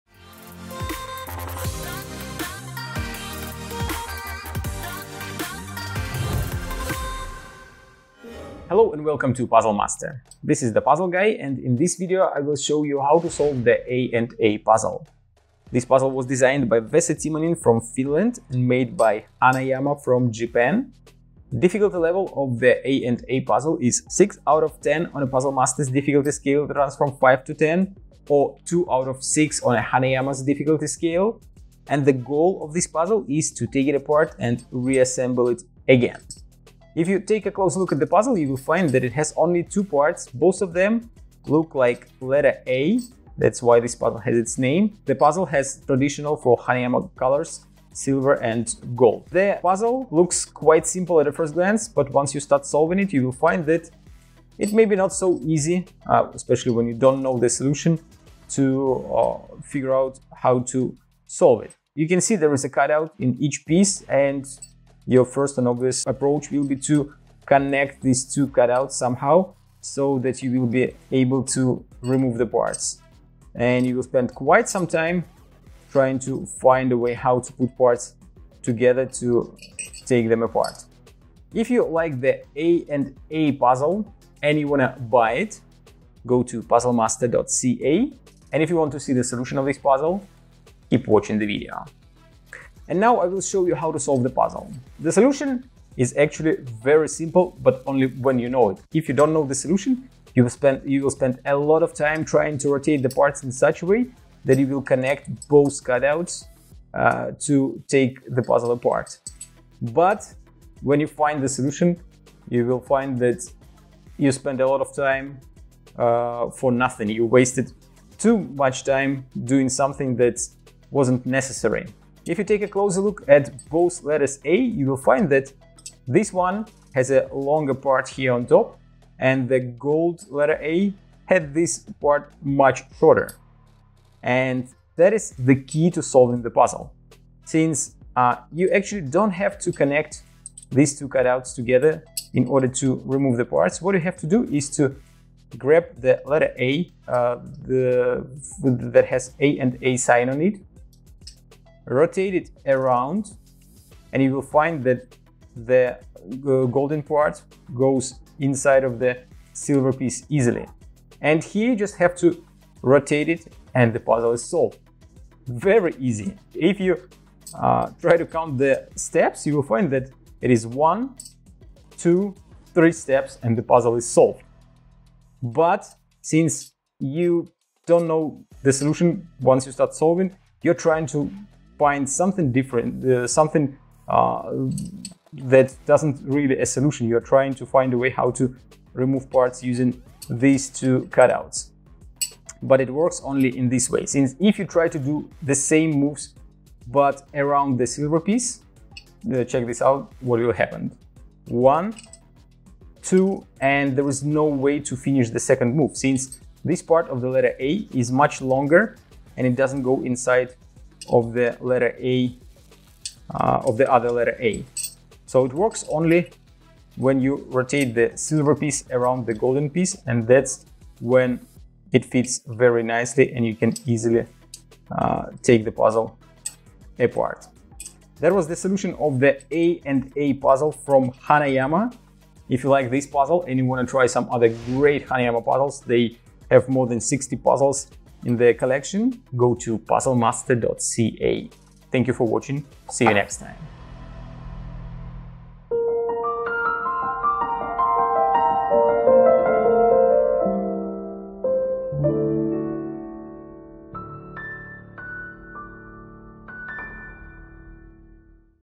Cast a&a from Hanayama - Solution
How difficult can it be to get 2 pieces apart? When you look at the Cast a&a from Hanayama, there is an obvious looking solution.

The movement is simple, but at the same time sophisticated. Just because you solved it once, doesn't mean it will be easy the second time.  It is a puzzle that you can play with over and over again. The Hanayama Cast Huzzle series provides puzzles for adults which are solved using a blend of inspiration and logic.